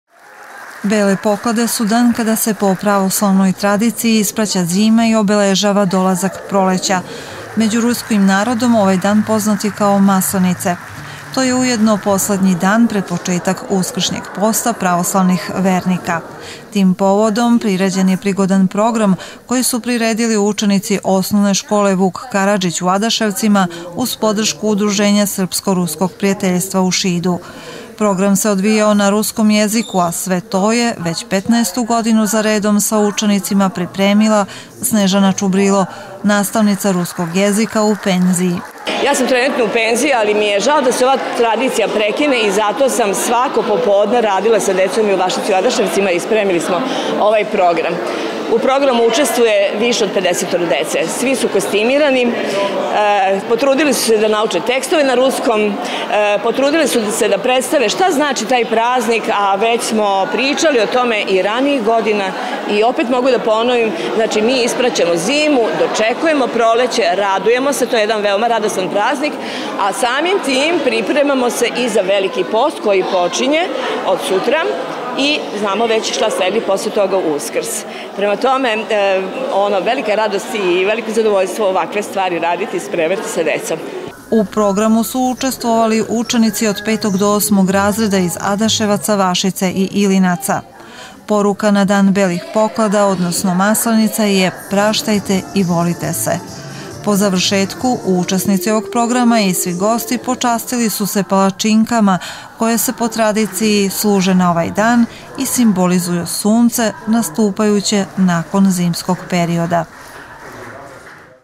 Bele poklade
U sportskoj sali Osnovne škole „Vuk Karadžić“ u Adaševcima   juče je prikazan  prigodan   program  povodom Belih poklada odnosno Maslenica.  Sadržaj programa  pripremili su učenici ove škole uz podršku Udruženja srpsko ruskog prijateljstva Šid. Bele poklade su praznik kojim se obeležava kraj zime i doslazak proleća a kao simbol  sunca, domaćice pripremaju palačinke